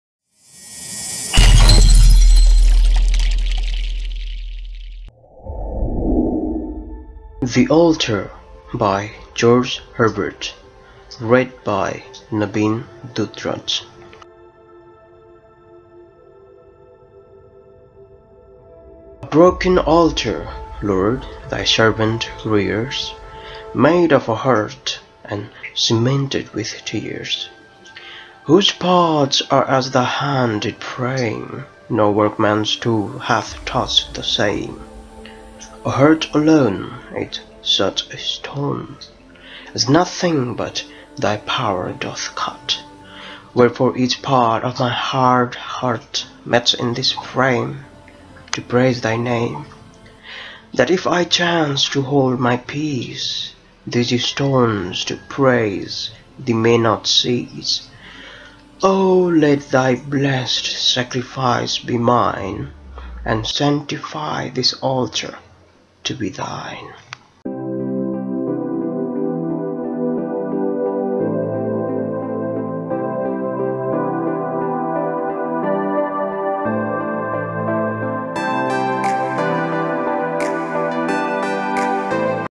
The Altar by George Herbert @ Epic Poetry @ 4k Video
The Altar
GEORGE HERBERT
(Epic Poetry)

A broken ALTAR, Lord, thy servant rears,
Made of a heart and cemented with tears;
Whose parts are as thy hand did frame;
No workman's tool hath touch'd the same.
A HEART alone
Is such a stone,
As nothing but
Thy pow'r doth cut.
Wherefore each part
Of my hard heart
Meets in this frame
To praise thy name.
That if I chance to hold my peace,
These stones to praise thee may not cease.
Oh, let thy blessed SACRIFICE be mine,
And sanctify this ALTAR to be thine.